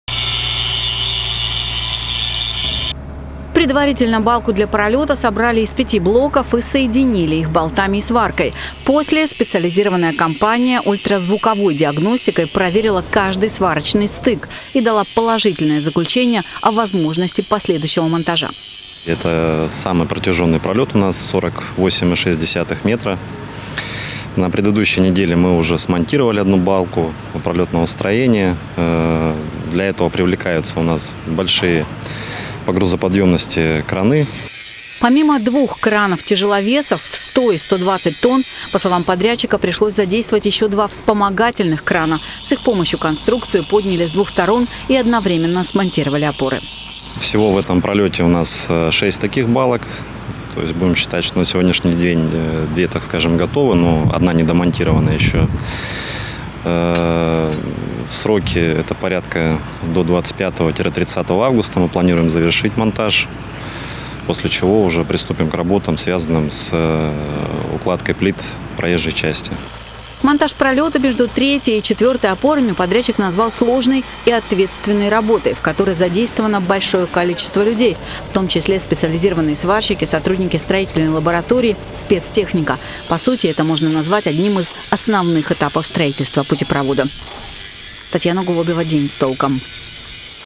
На мосту у Нового рынка в Барнауле смонтировали вторую балку самого протяженного пролета
Работы по монтажу балки между третьей и четвертой опорами велись ночью, в предоставленное железнодорожниками "технологическое" окно.